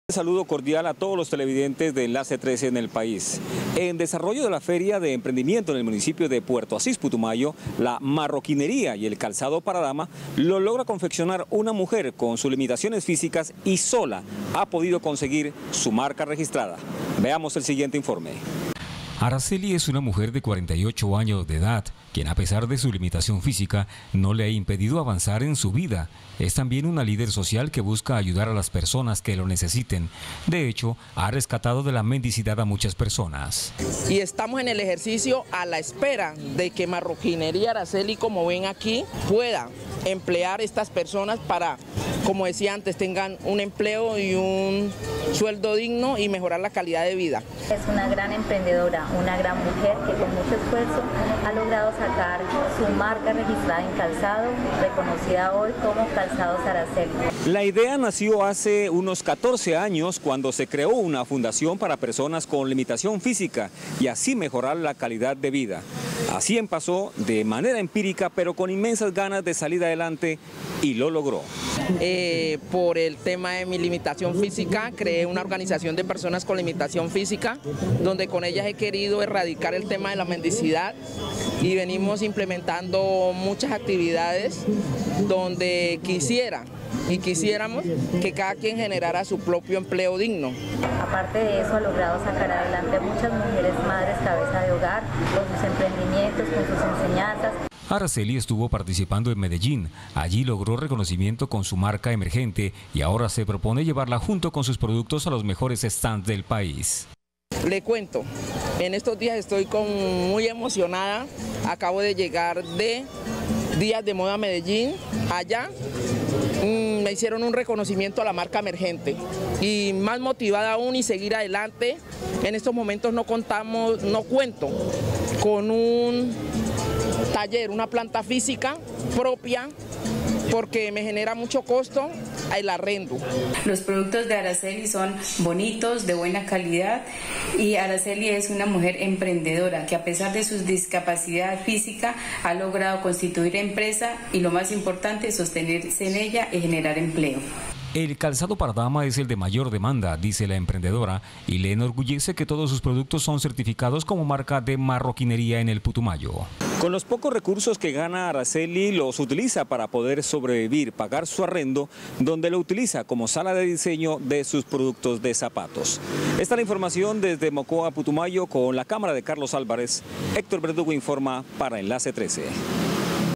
Limitación física no impidió a una mujer crear zapatos y registrar su marca | Noticias Enlace Trece
🟠📺 EnlaceTrece | Una mujer en Putumayo encontró en la confección de calzado, la oportunidad de tener su pequeño negocio 🥿 Comenzó sola, haciendo todo el proceso de cortado y elaboración. Con el tiempo, y la llegada de la pandemia, ha tenido que abrirse paso en medio de un terreno competido, pero la suerte la ha acompañado y ya puede generar empleo, aunque su emprendimiento sigue posicionándose.